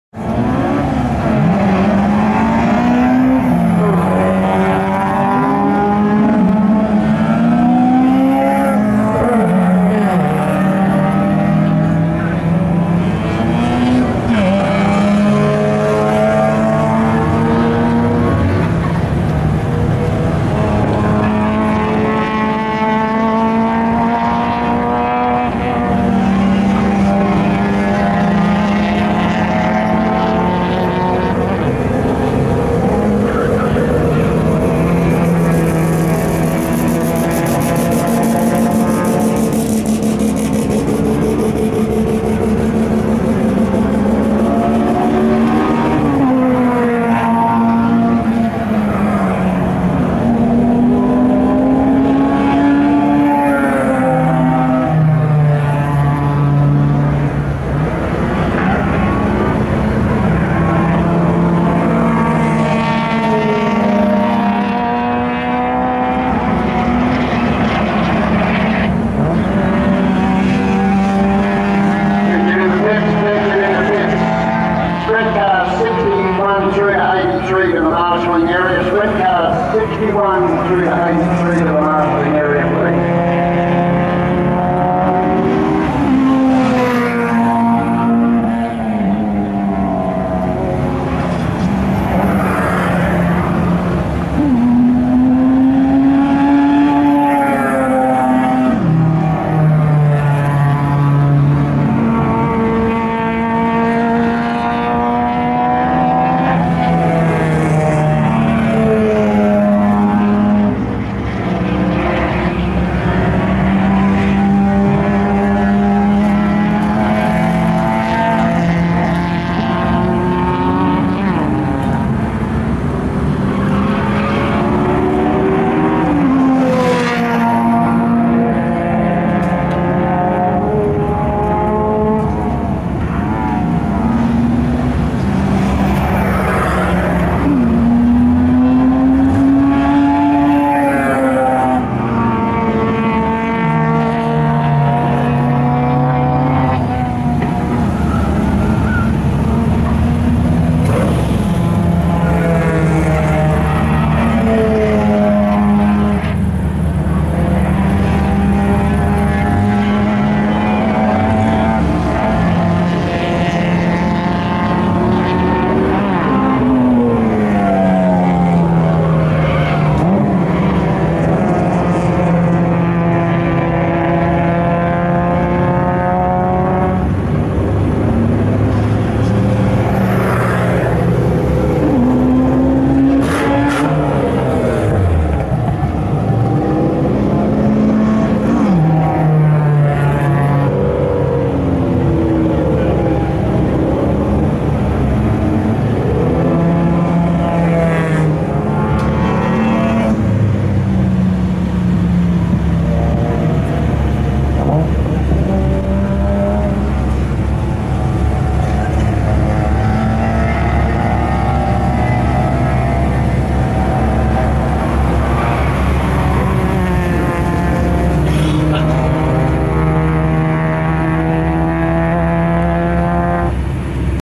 SCMSA Rd2 QLD v NSW Lancer v RX7
Having a good run against a Mazda Rx7 at Morgan Park Raceway for Rnd2 of the NSW v QLD Sprint Series